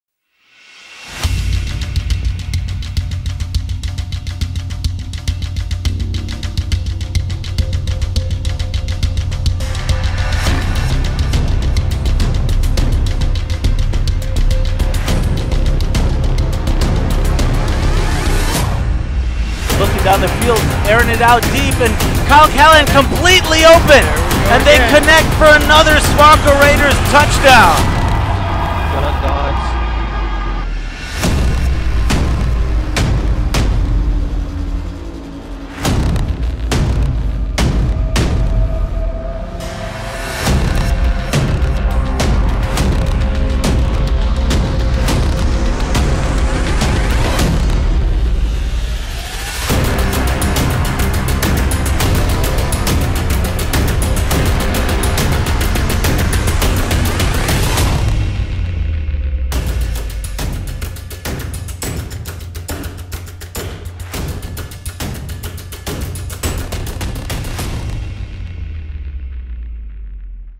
Trailer SWARCO Raiders Tirol vs Schwaebisch Hall Unicorns: Big6 2016
The SWARCO Raiders Tirol will host the Schwäbisch Hall Unicorns on May 7th  in the Big6 tournament of 2016.
Schwäbisch Hall ist the former team of Moritz Boehringer, who got drafted by the NFL team Minnesota Vikings in the 5th round of the 2016 NFL draft.

So two of the best American Football teams in Europe will battle each other and maybe we see the next players, who will be going to the NFL in this game! So tune in on May7th at 17:10cet / 11:10ET. Kickoff is around 18:00cet / noon ET.